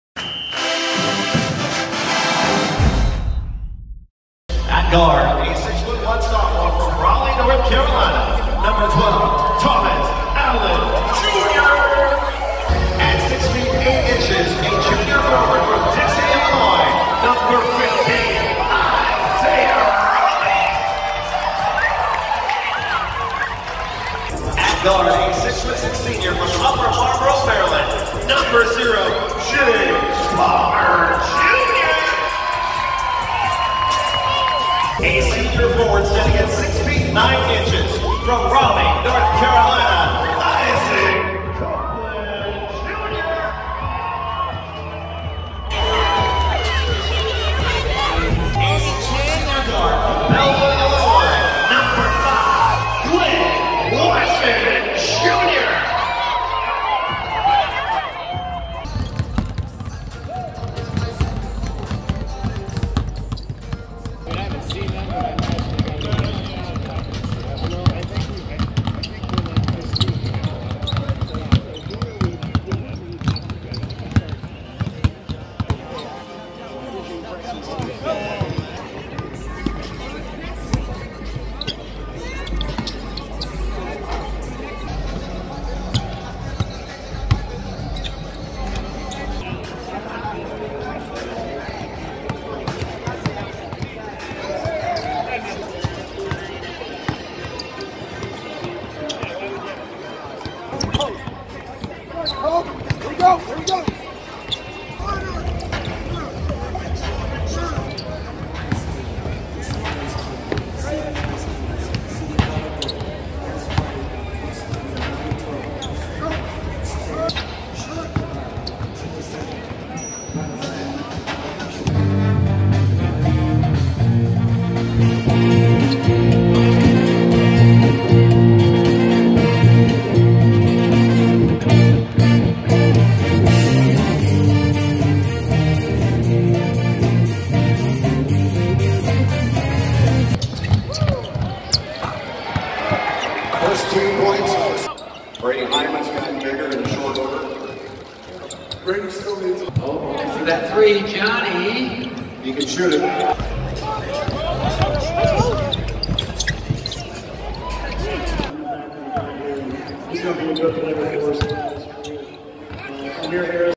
Nebraska Basketball: Sights and Sounds from the Husker Hoops Preview
Nebraska basketball hosted an open practice Wednesday night at Pinnacle Bank Arena in anticipation of the 2018-19 season.

Hail Varsity was on hand to catch all of it.